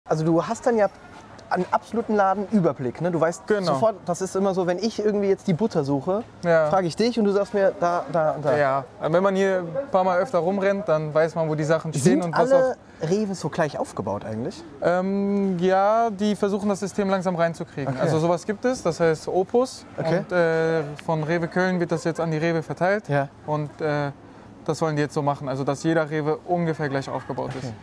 Sind alle Rewe gleich aufgebaut?!
Ist es euch auch schonmal passiert, dass ihr in einer anderen Stadt im Supermarkt minutenlang durch die Gänge gelaufen seid, um eure Lieblingsschokolade zu finden? Und ihr euch gefragt habt, wieso nicht alles gleich aufgebaut ist? Genau daran arbeitet REWE mit dem System OPUS!

Falls ihr euch für den Beruf zum:zur Kaufmann:frau im Einzelhandel interessiert schaut doch einfach mal in unsere komplette smusy.meets Folge rein!